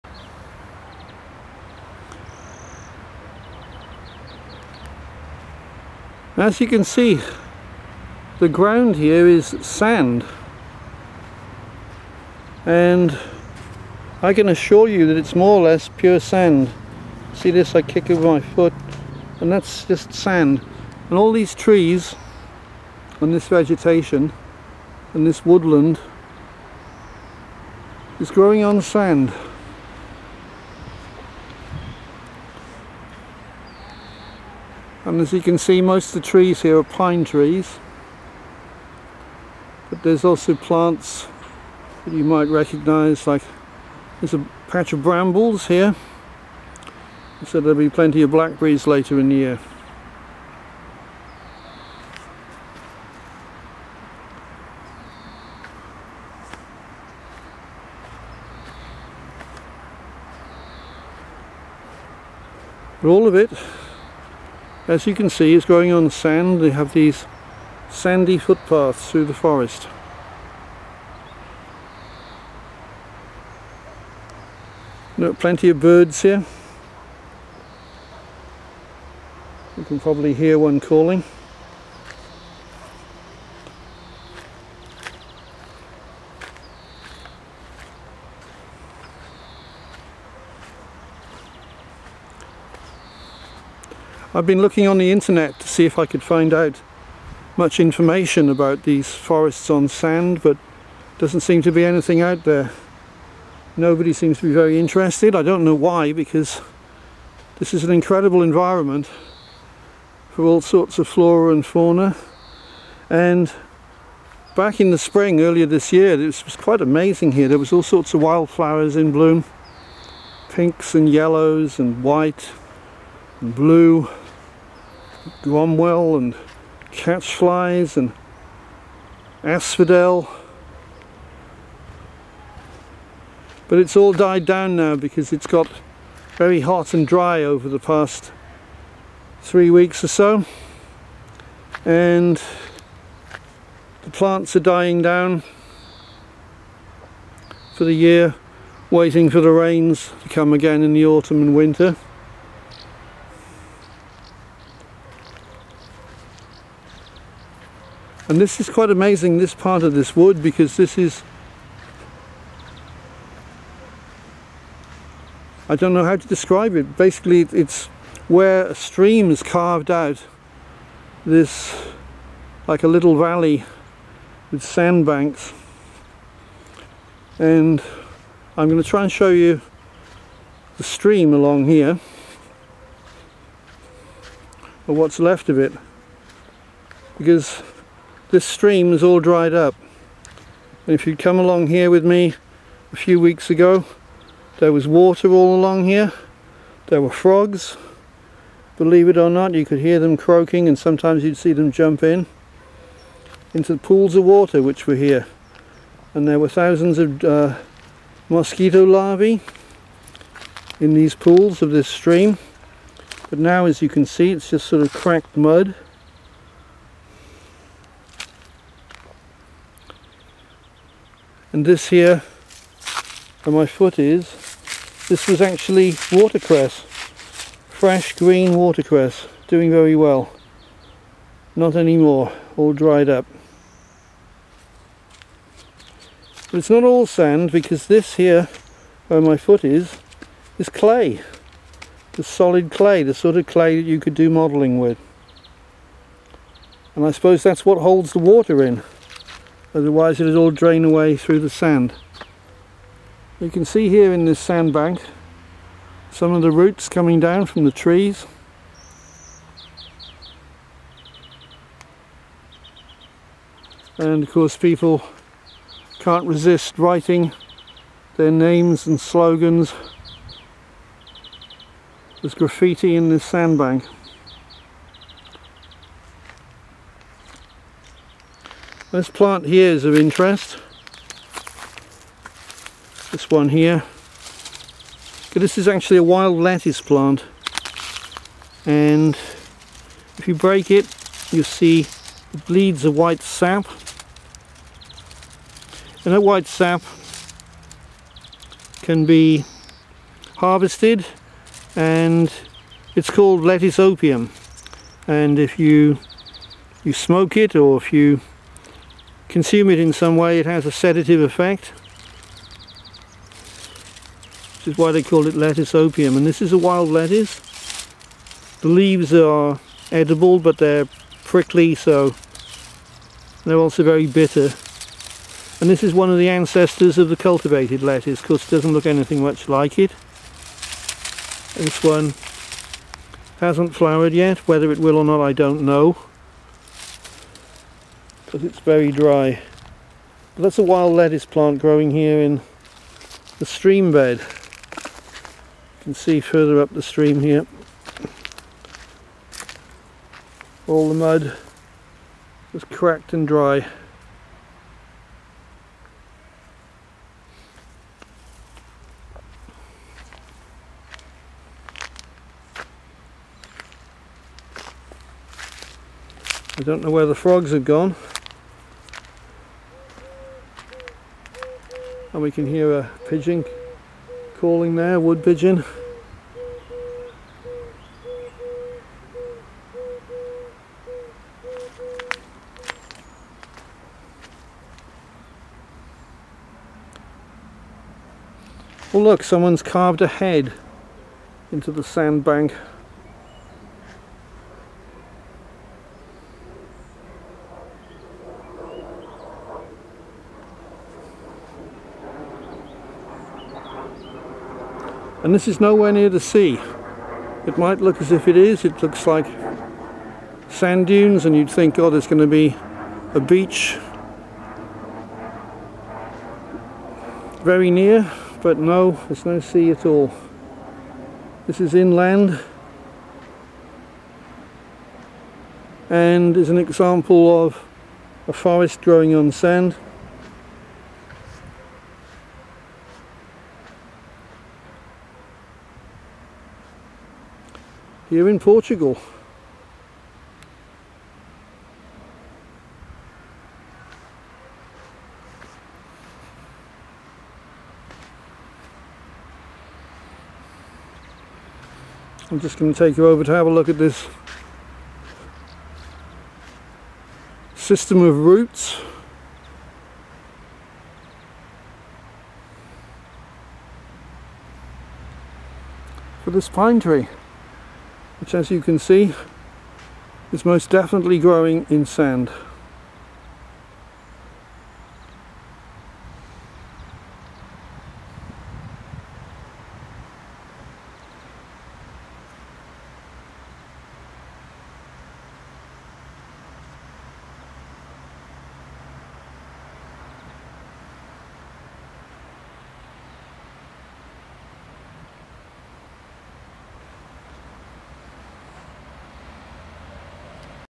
Portugal's pine forests growing on sand
There are small forests in Portugal growing on sand and surrounded by sandy heaths. Many species of the Portuguese flora and fauna live in these habitats. The trees are mostly pines and cork oak.  Streams may dry up but when there is water in them they support frogs and plants like the watercress. Wild lettuce is one plant that grows in such places. The forests are a long way from the sea and growing inland between Lisbon and Setubal..